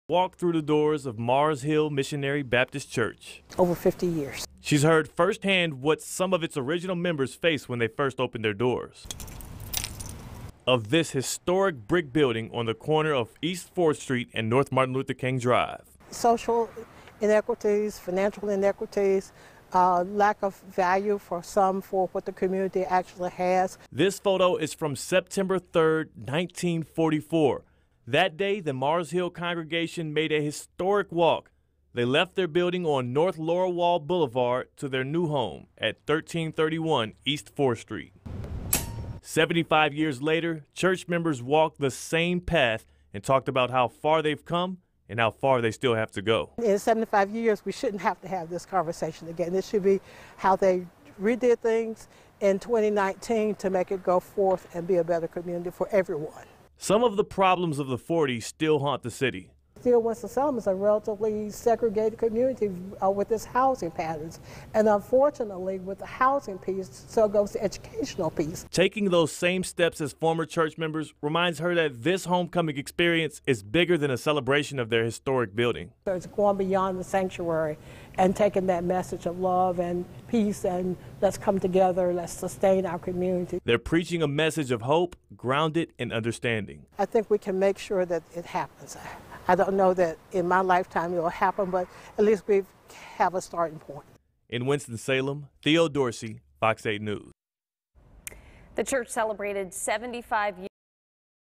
Mars Hill Missionary Baptist Church take historic walk
Church members walked the same path and talked about how far theyve come and how far they still have to go.